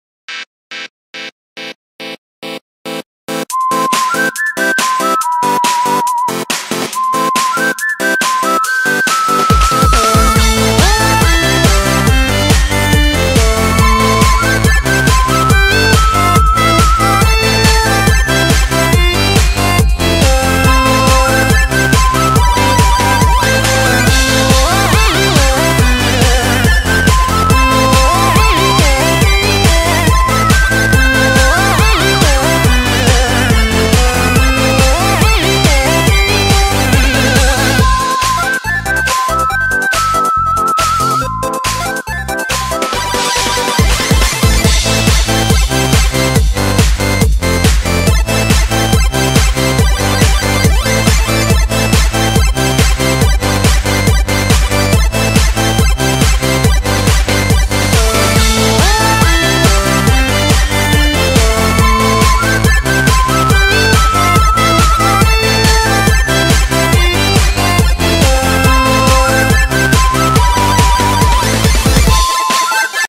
[4K 60fps] Byte System by MoonBlade2000 (3 Coins)
Level Info:

Level Name: Byte System
Level ID: 13341185
Level Author: MoonBlade2000
Level Difficulty: Insane 8★